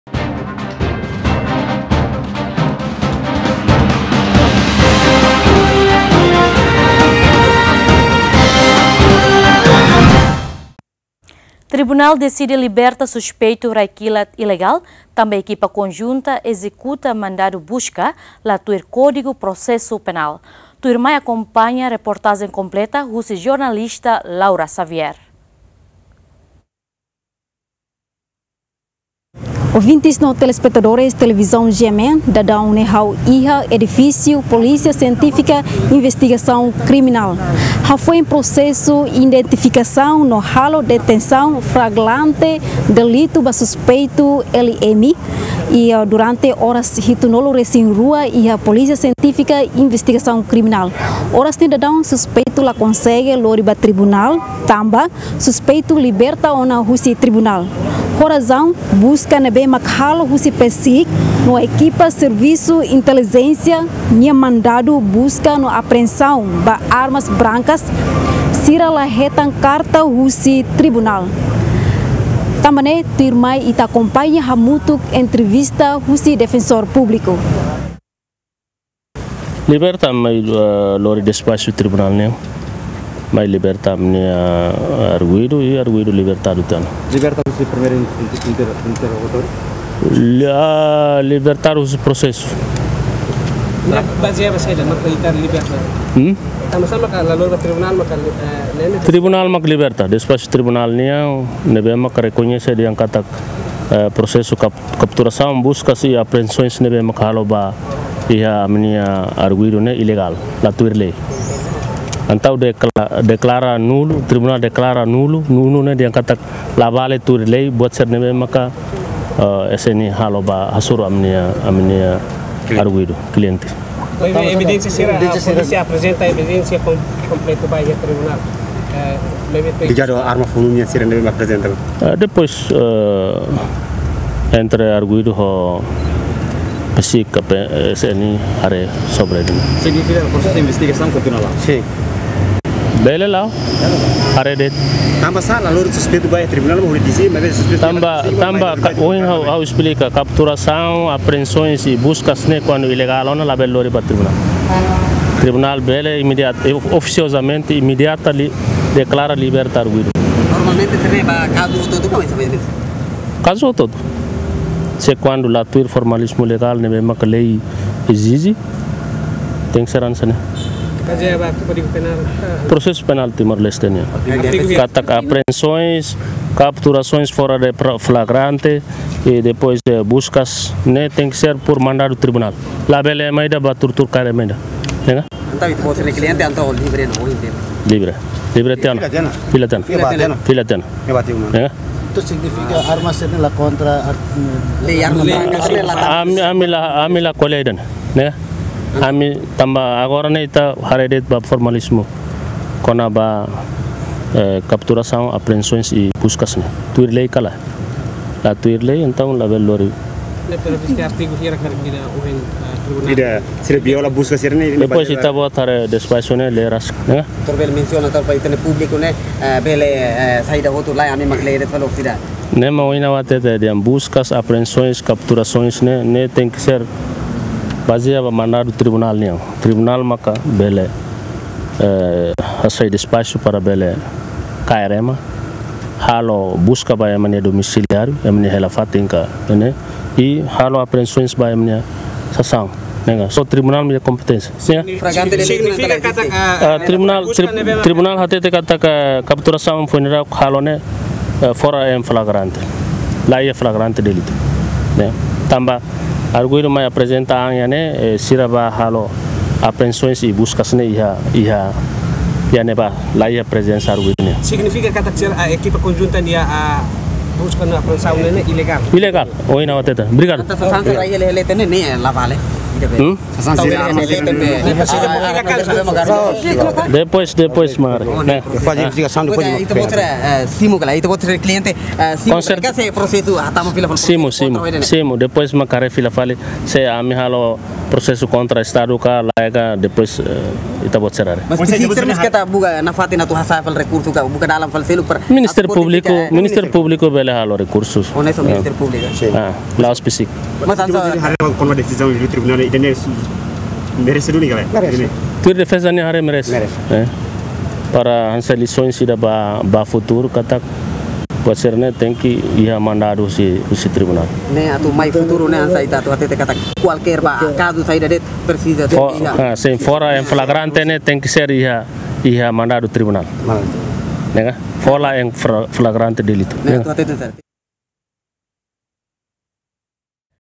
Liberta suspeitu LM tanba ekipa konjunta ezekuta mandadu buska la tuir kódigu prosesu penal
TDD desidi liberta suspeitu rai kilat ilegál tanba ekipa konjunta ezekuta mandadu buska la tuir kódigu prosesu penal